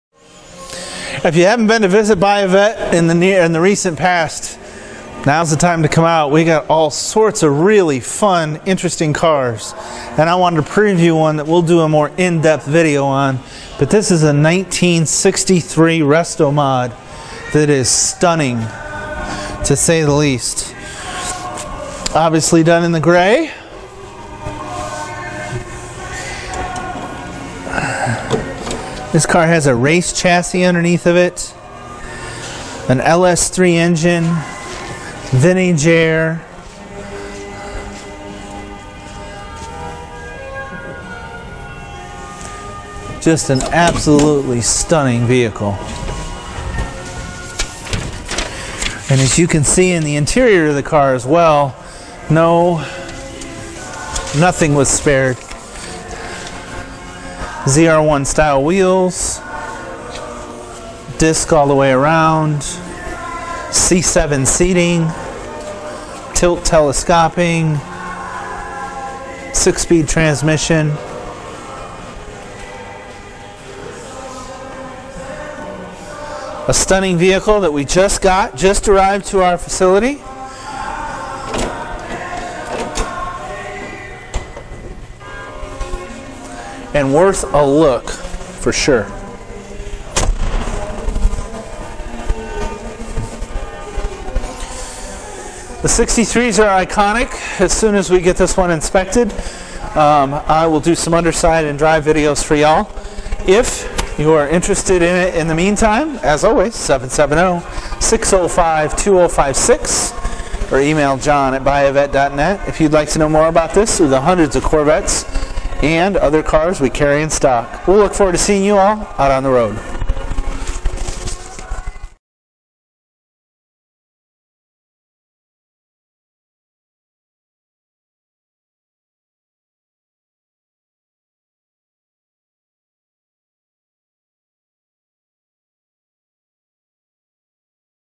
1963 Corvette LS3/5 Speed Split Window Coupe Restomod
1963 Corvette LS3/5 Speed Split Window Coupe Restomod For Sale

Gray exterior with Black hood stinger, Kalahari leather interior. 450hp LS3 engine, Tremec TKO600 5 speed manual transmission, 3.42 posi rear-end. Features include air conditioning, power steering rack and pinion steering, chrome C6 ZR1 style wheels, redline radial tires, 4 wheel disc brakes, tilt, power windows, power locks, dual power genuine leather seats, AM/FM/BT radio, keyless entry and start. Originally assembled on October 4, 1962, and rebuilt to its current custom state in late 2014, this 1963 Corvette Split Window Coupe features a custom Street Shop frame, C7 Z51 suspension, LS3 crate engine, 5 speed manual transmission, and a TON of comfort and convenience features that you'd never find in an original '63 Corvette! This extremely high quality build features vg-exc paint, with a gorgeous shine. Chrome bumpers and exterior door handles are vg-exc. A 1967 big block stinger style hood has been added, and weatherstripping and taillights are vg-exc. Added chrome C6 ZR1 style wheels wear a set of Nitto Invo tires with added redlines, which show an average of 9/32nds tread depth remaining. The interior of this 1963 Corvette Split Window Coupe Restomod shows C7 seats, custom leather door panels, vintage style AutoMeter gauges, leather-wrapped dash pods, cutpile carpet, custom rear mat and center console, and a leather/polished steering wheel with billet aluminum tilt steering column. Other additions to this car include QA1 coilover shocks, aluminum radiator, electric fan, Borla exhaust, cold air intake, polished a/c compressor, polished radiator hose covers, and window tint. Invoice from Street Shop Inc. is included. This car has been fully serviced, and all instrument gauges, lights, turn signals, brakes, engine, transmission and rear-end are all in good operating condition.